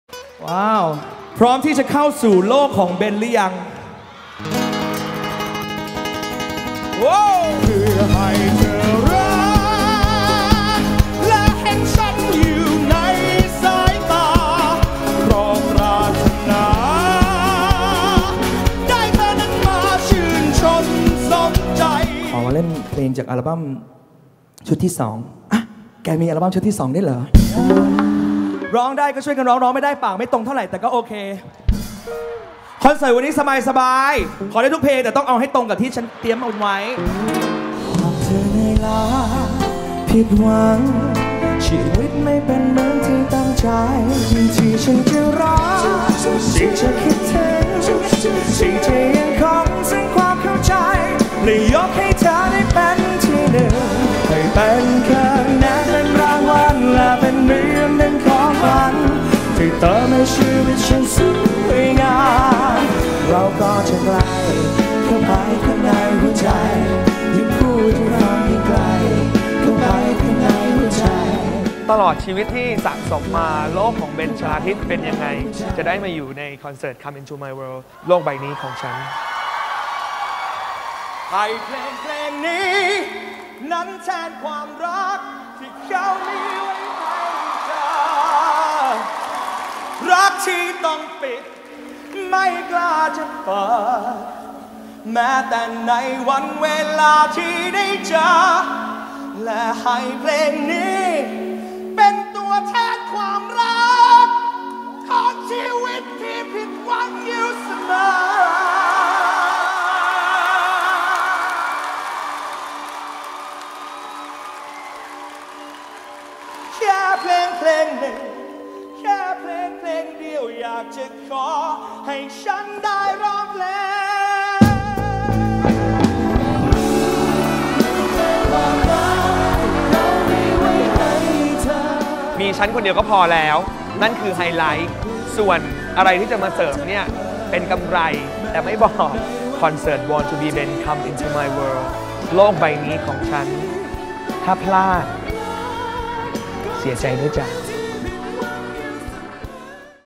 Born to be Ben - Come into my World โลกใบนี้ของฉัน
25 กรกฎาคมนี้ มาเปิดโลกของคุณ เพื่อต้อนรับเข้าสู่โลกของ “เบน ชลาทิศ” ในคอนเสิร์ตใหญ่ “Born to be Ben - Come into my World โลกใบนี้ของฉัน” ปลดปล่อยกันให้สุดพลัง สนุกสุดเหวี่ยงได้ไม่มียั้ง จัดเต็มเสื้อผ้าหน้าผมมาได้เลย! ใครยังไม่มีบัตรหาซื้อได้แล้ววันนี้ ที่ ไทยทิคเก็ตเมเจอร์ทุกสาขา โทร. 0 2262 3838 หรือ

แล้วเจอกันที่อิมแพ็ค อารีน่า เมืองทองธานี นะจ๊ะ